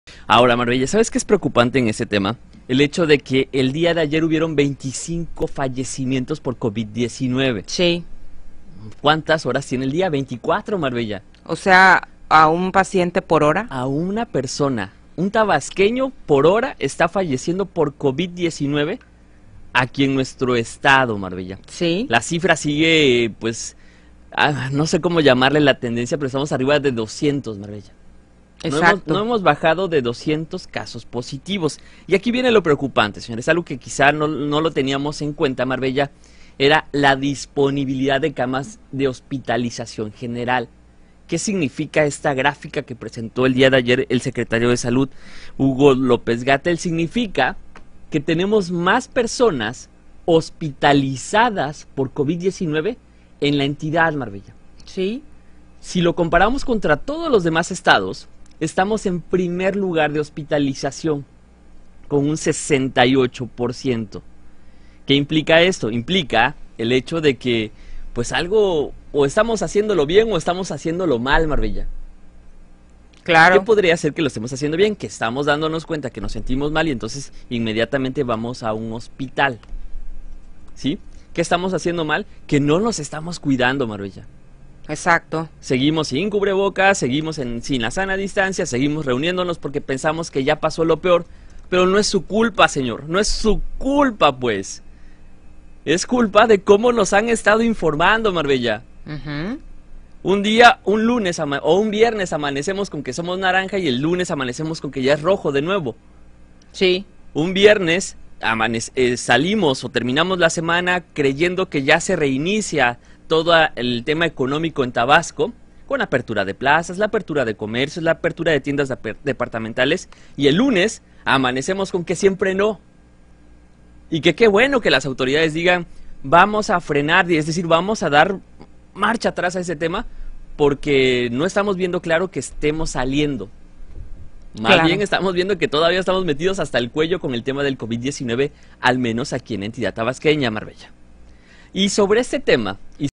Tabasco primer lugar nacional en ocupación de camas de hospitalización general y solo el día de
ayer fallecieron 25 tabasqueños (01/07/2020), prácticamente una persona por hora. ¿Realmente estarán funcionando las estrategias de contención?  Cuídate y cuidémonos todos.